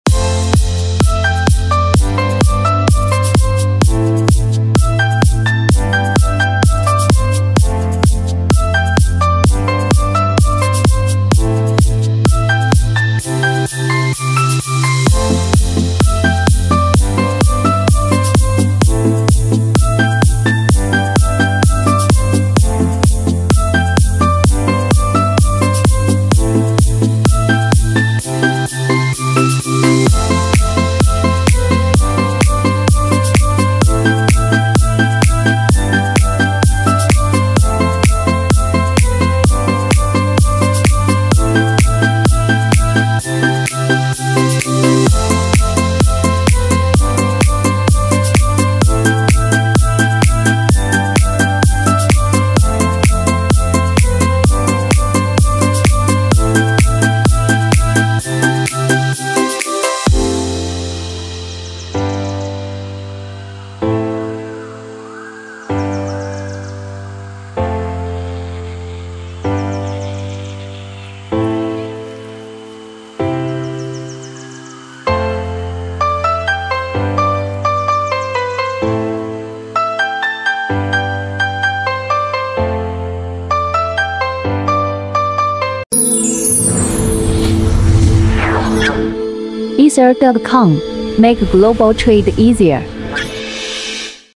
Heat Automatic Shrink Packaging Machine 2.5KW POF Film Fully Sealed
We can provide Heat Automatic Shrink Packaging Machine，2.5KW Automatic Shrink Packaging Machine，POF Film Automatic Shrink Packaging Machine and so on.
Qingdao Sanweihe Machinery Manufacture Co., Ltd., quality Heat Automatic Shrink Packaging Machine 2.5KW POF Film Fully Sealed manufacturer from China.